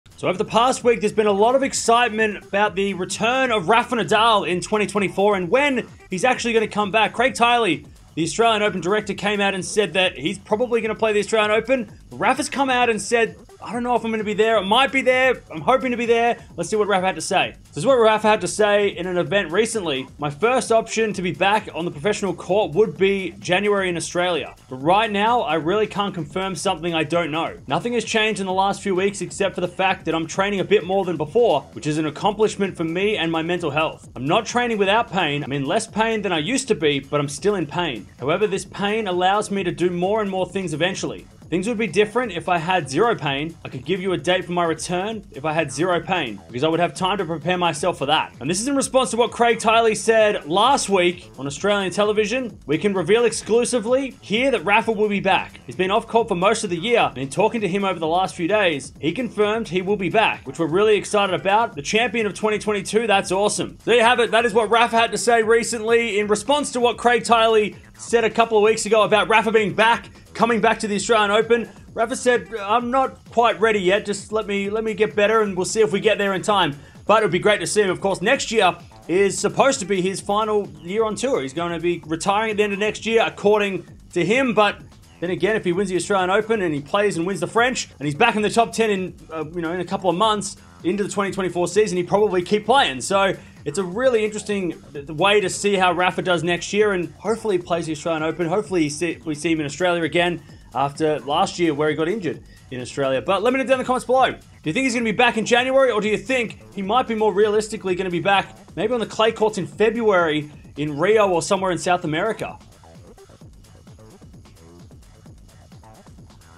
Nadal Unsure of Australian Open 2024 Return | Tennis Talk News
Rafael Nadal has thrown some doubt on his return at the start of next year at the Australian Open 2024 saying in an interview he was still feeling some pain and can not set a return date when he is still in pain. When will Rafa return?

0:00 | Intro
0:17 | Nadal Interview
0:52 | Tiley Interview
1:03 | Outro